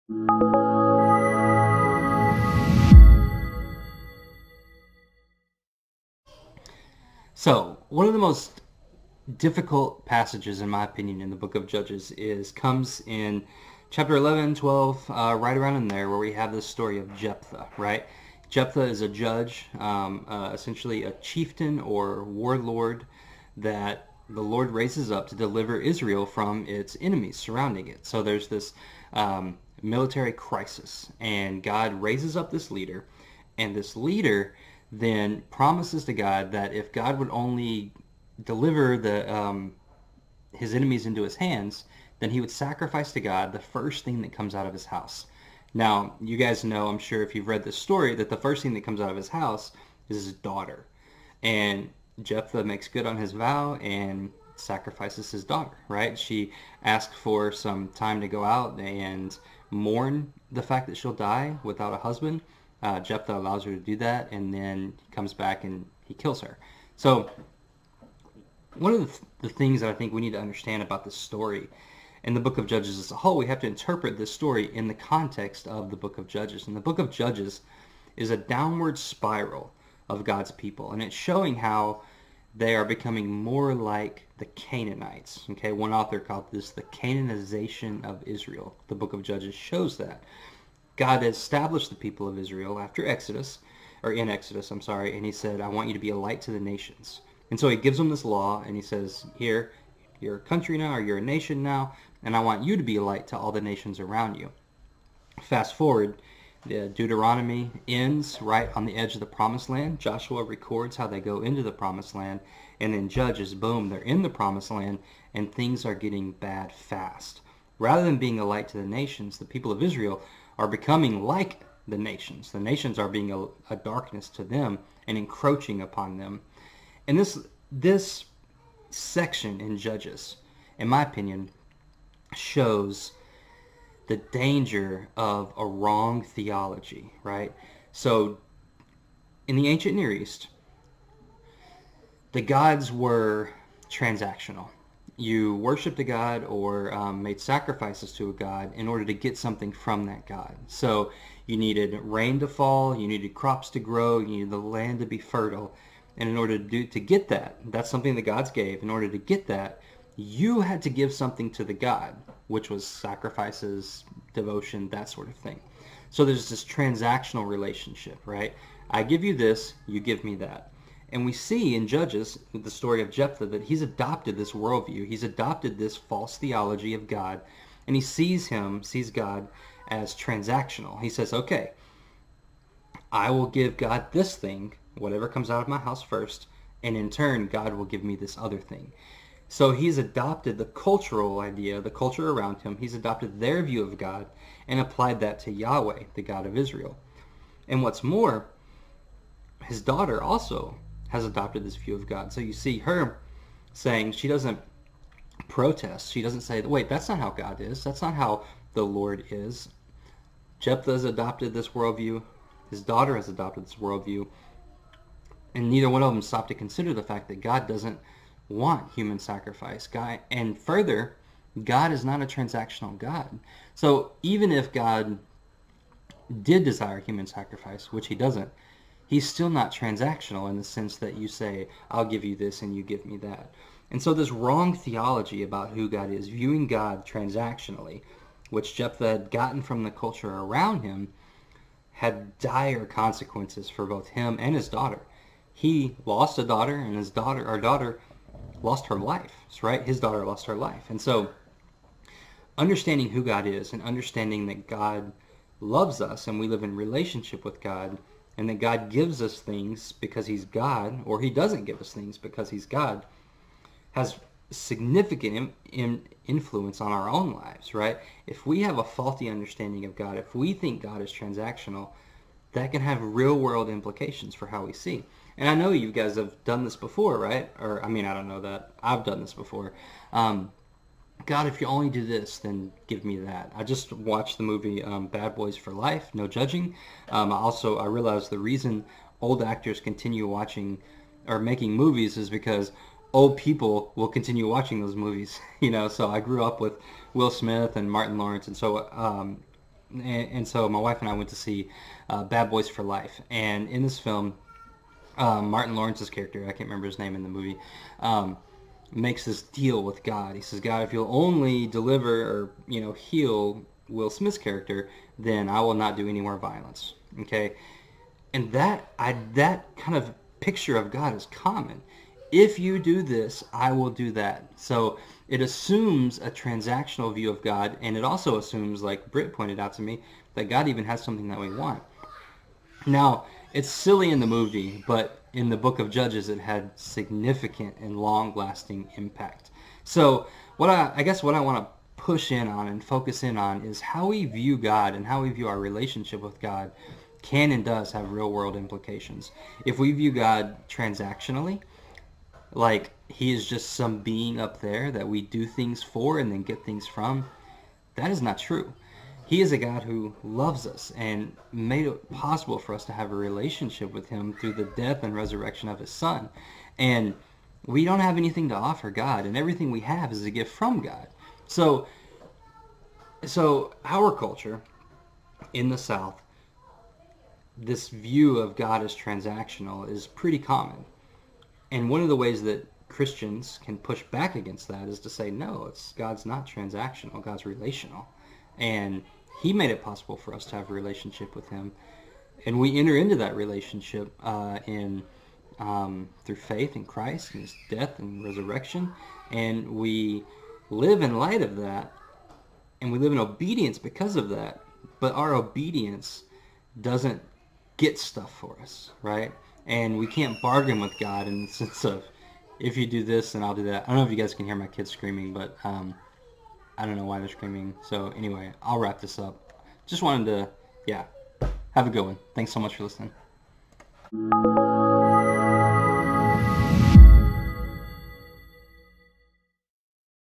Jephthah and the Consequences of Bad Theology (Old Testament Explainer)
What in the world is going on with Jephthah killing his daughter? In this video I try to work through some of the dangers of a wrong view of God.